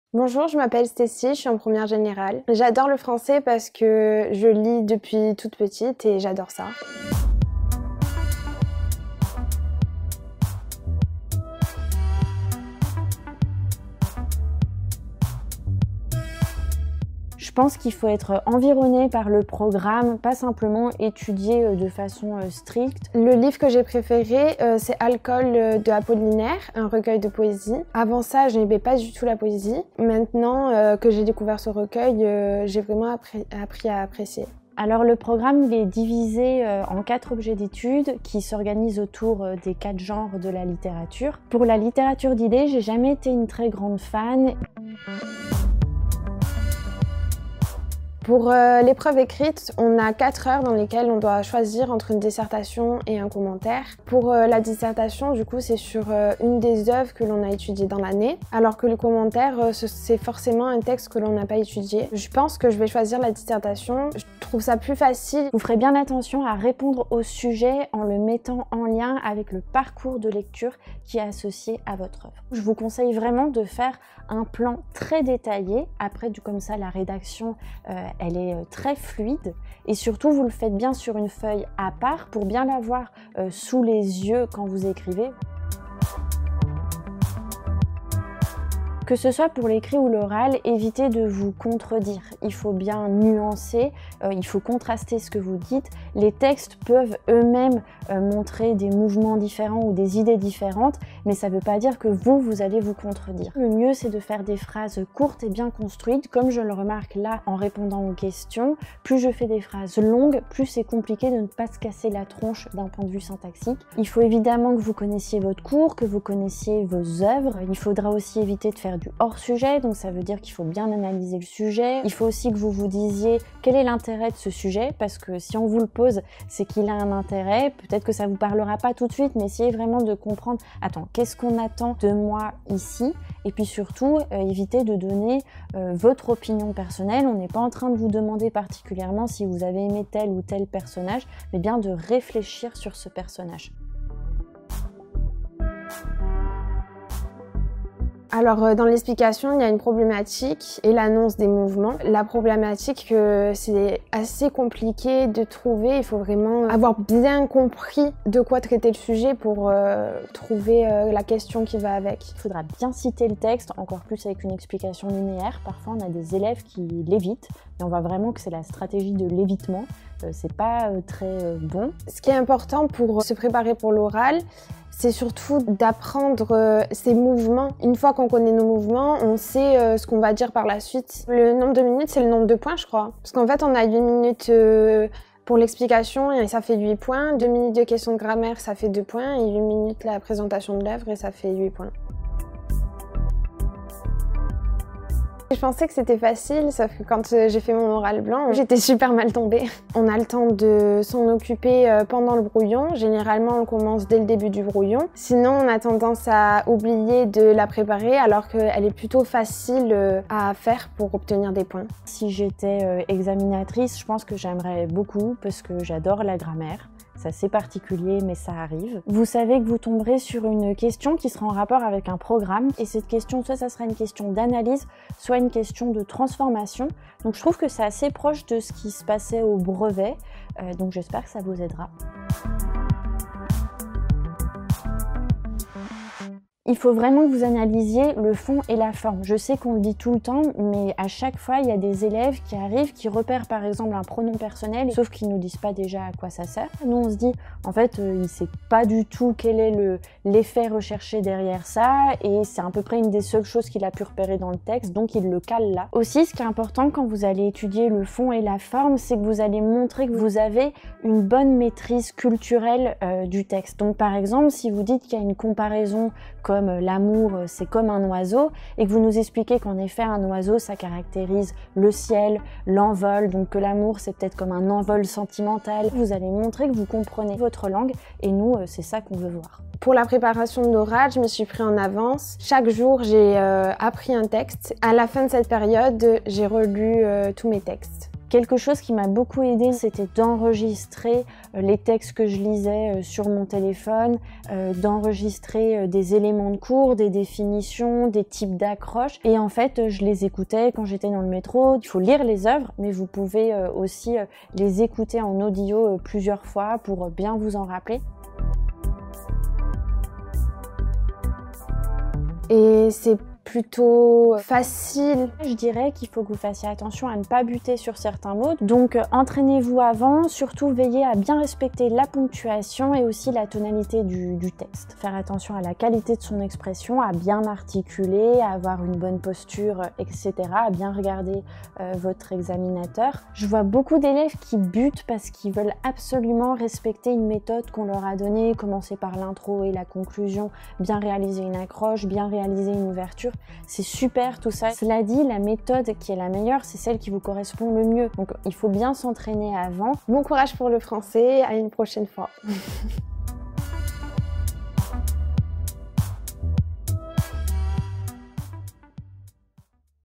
Bac de français : ça se passe comment ?
Bac de français : comment se préparer ?
Le bac de français est approche ! Révise les épreuves écrite et orale de français avec des conseils de révisions ! Maîtriser les objets d'étude, la dissertation et le commentaire, connaître les œuvres au programme et les parcours de lecture, s'entraîner pour l'oral : pour tout savoir et préparer les épreuves anticipées du bac, regarde notre vidéo !

00:00 - Introduction
00:12 - Programme de français en première
00:45 - Bac de français : modalités
02:24 - L'oral de français : comment ça se passe ?
03:12 - La question de grammaire : oral de français
03:52 - Oral du bac de français : tous nos conseils !
05:09 - Réussir la lecture à voix haute à l'oral de français